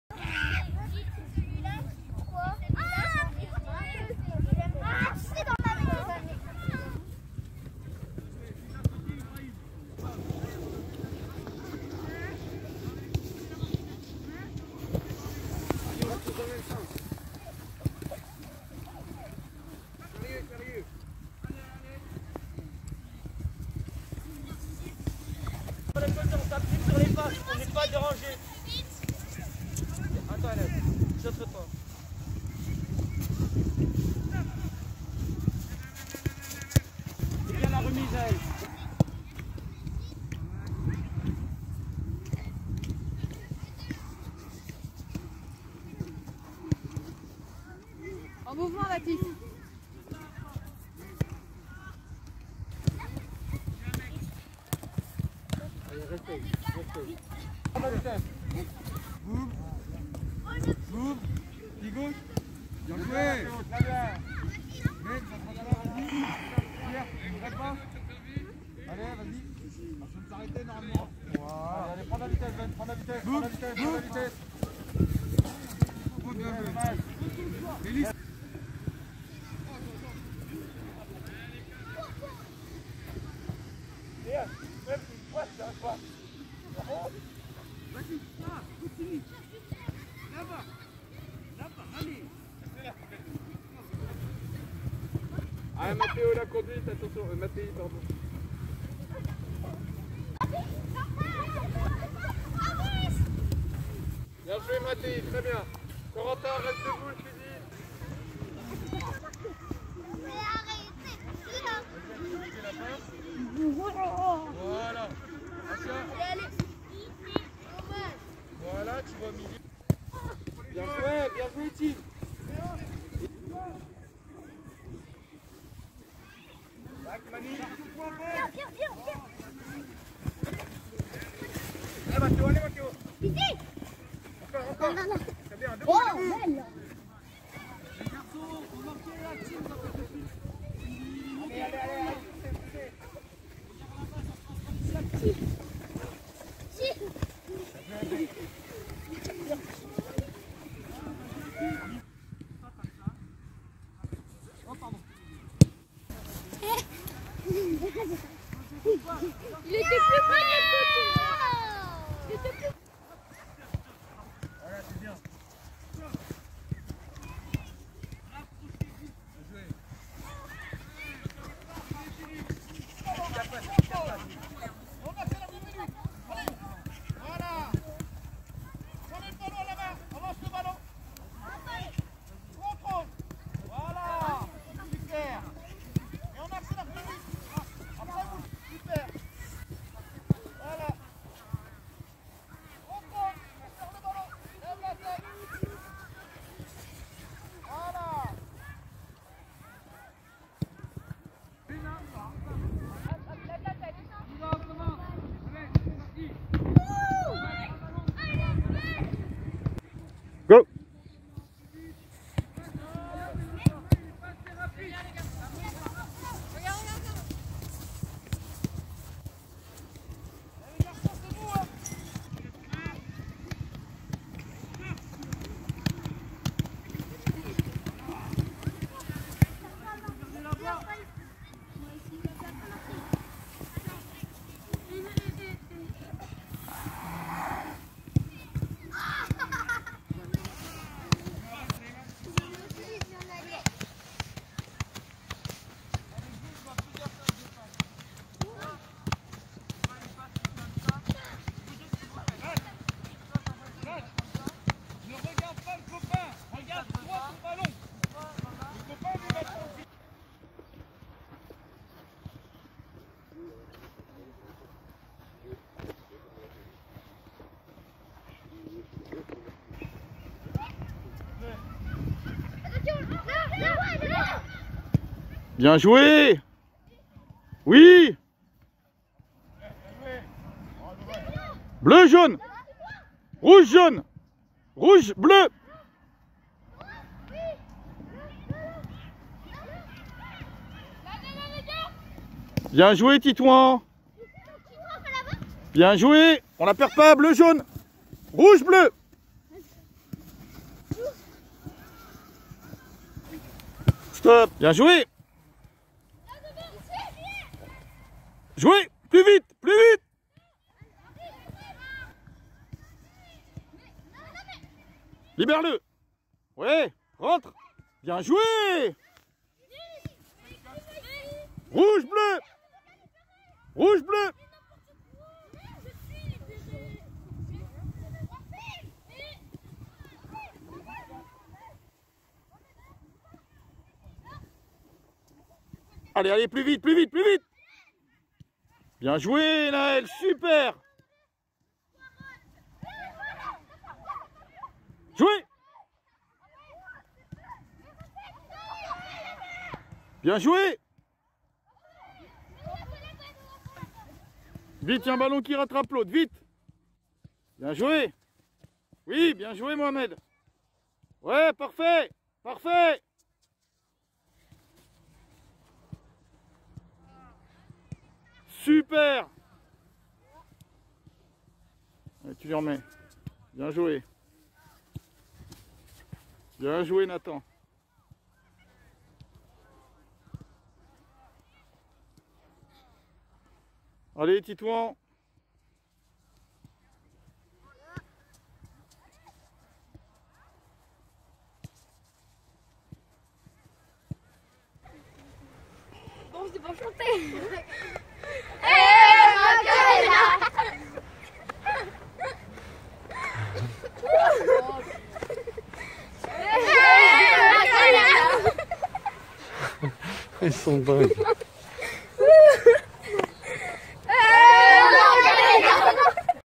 2021-01-27 Images prises "à la volée"
Extraits des entrainements U7 à U11 du mercredi 27 janvier 2021
petite danse des U11F en fin de vidéo